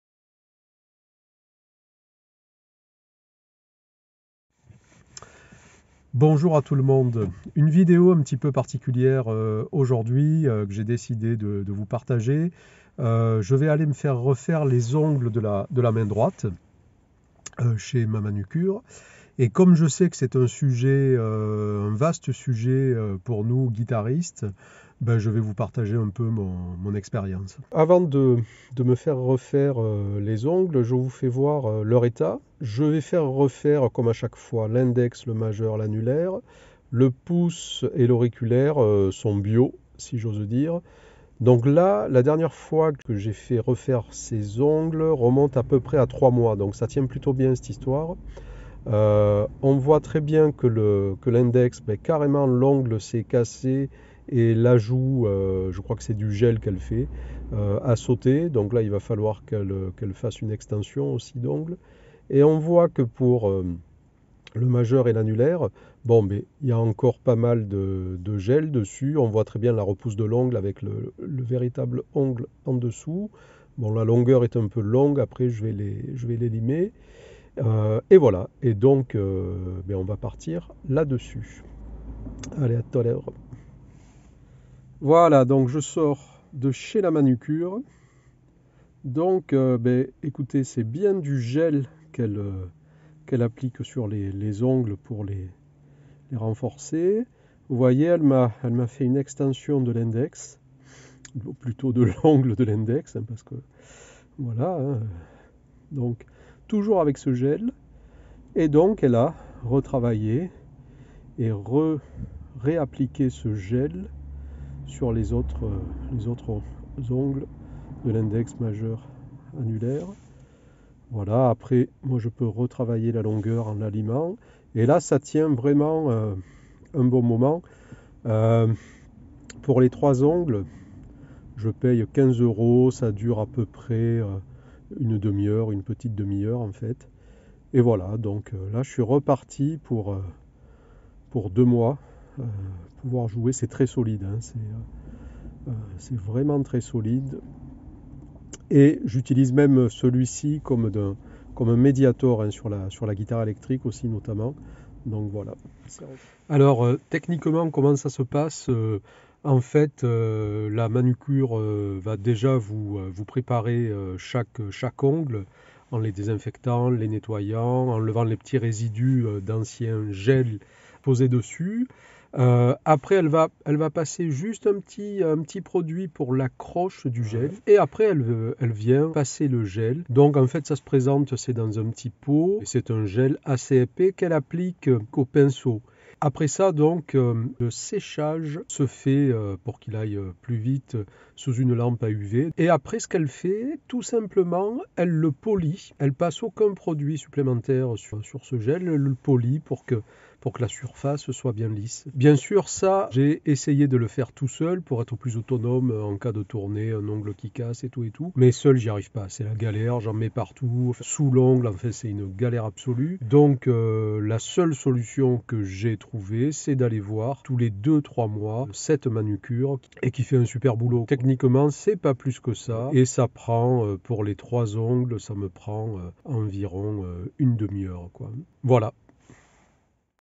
Ongles Guitare et Manucure
Quelle technique utiliser pour faire ou refaire ses ongles ?!!!
Voilà une question que se posent fréquemment beaucoup de guitaristes.
Personnellement, après avoir essayé les faux ongles, la poudre acrylique ... j'ai opté pour l'utilisation du gel, qui me renforce les ongles et permet également de les reconstituer quand ils se cassent.
Je vais donc, tous les 2 mois chez une manucure qui me pose, à l'aide d'un pinceau ce fameux gel.

La chaine Youtube de Jean Marie FREDERIC : guitare-jazz-pro